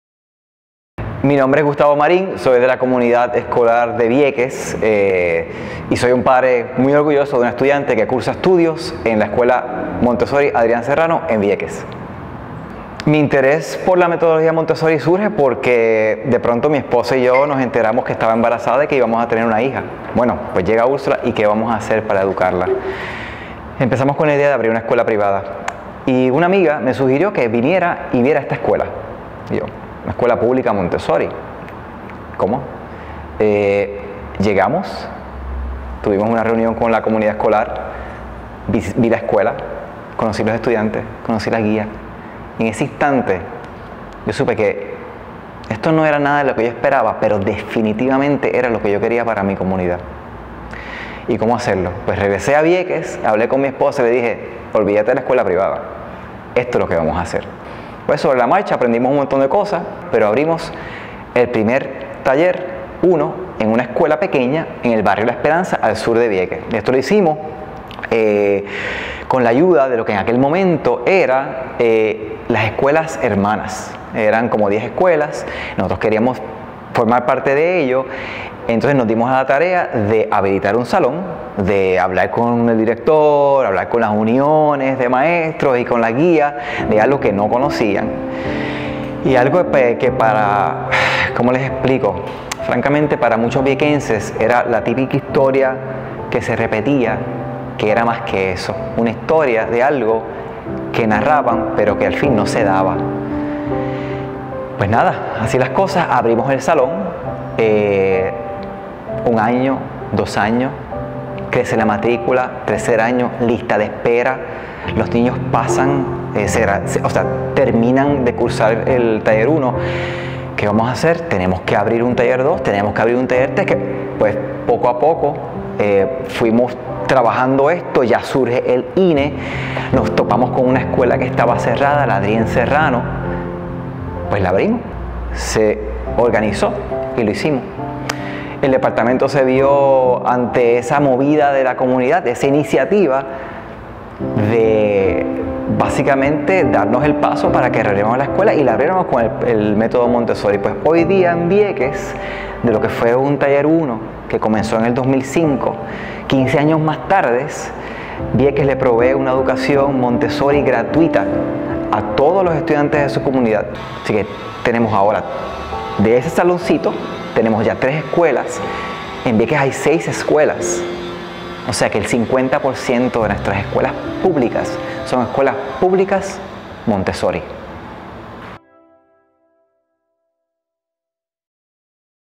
Montessori Público en Vieques Puerto Rico
Un padre de la comunidad nos cuenta como es que Montessori Público llega al municipio de Vieques ,y ahora el 50% de las escuelas públicas de dicho municipio son Montessori, pero gratuitas.